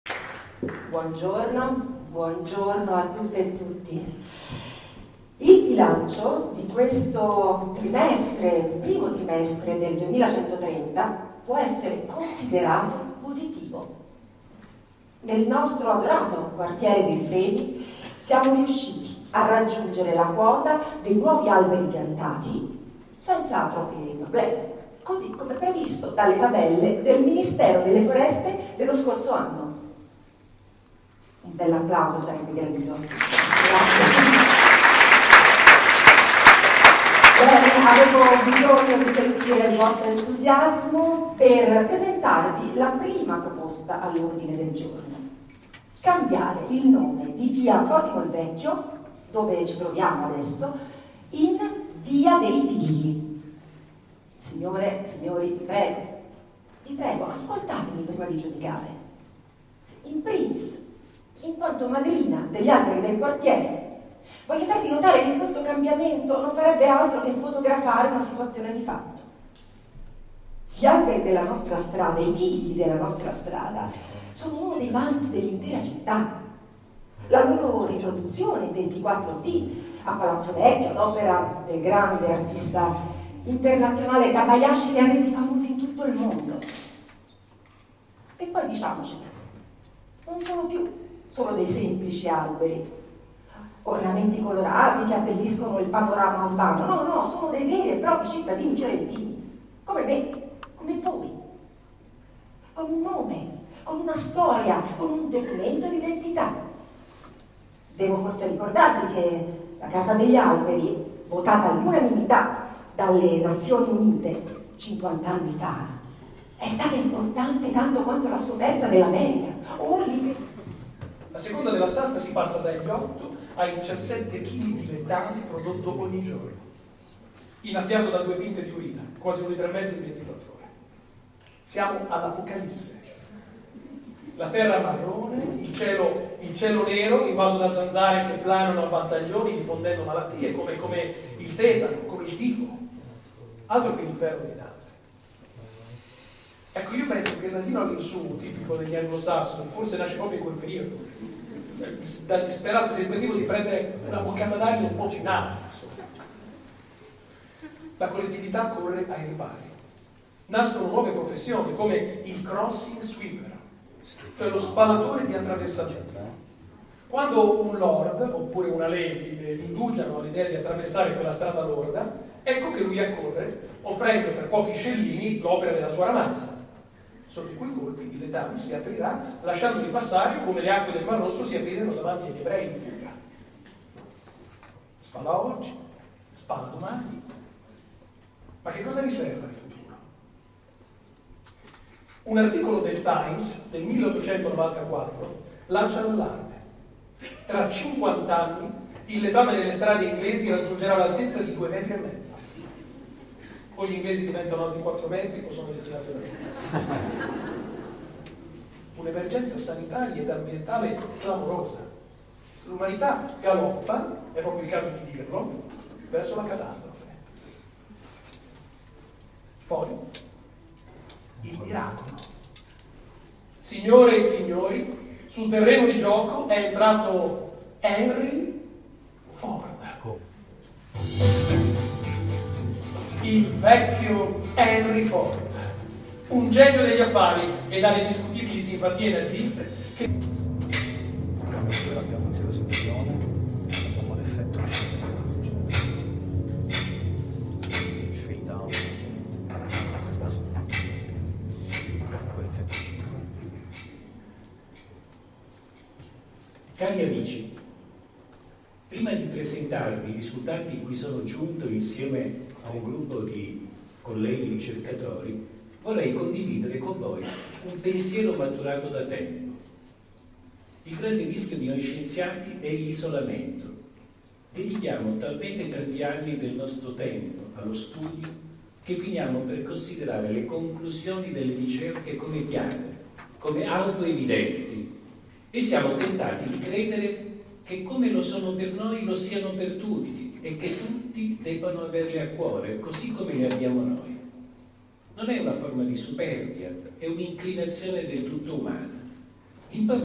PLANETARIA - Discorsi con la terra
Si è aperta ieri sera al Teatro della Pergola di Firenze con grande successo di pubblico la prima edizione di "Planetaria. Discorsi con la Terra", il nuovo format sul climate change che coniuga arte e scienza, ideato da Stefano Accorsi e Filippo Gentili e curato e prodotto da Superhumans con Fondazione Teatro della Toscana. Tutto sold out per la prima "Conferenza Immaginaria" sul tema TERRA che ha visto sul palco del Teatro della Pergola il Direttore artistico Stefano Accorsi, accompagnato dall'attrice Vittoria Puccini e dal botanico Stefano Mancuso, consulente scientifico dell’evento.